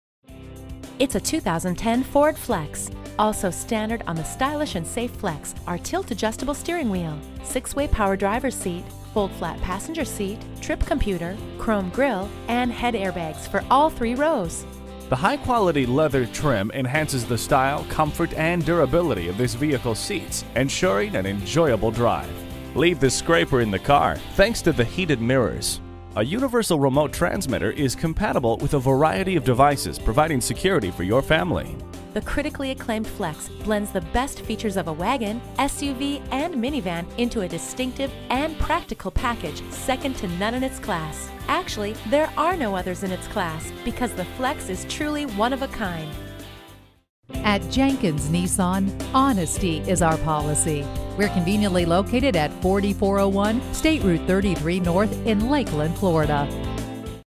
2010 Ford Flex Lakeland, FL FP738 - SOLD
Year: 2010
Make: Ford
Model: Flex
Trim: 4dr SEL FWD
Engine: 3.5 L V6 Cyl.
Color: SILVER
Mileage: 35507
Stock #: FP738

Jenkins Nissan
Address:
401 State Rd 33 N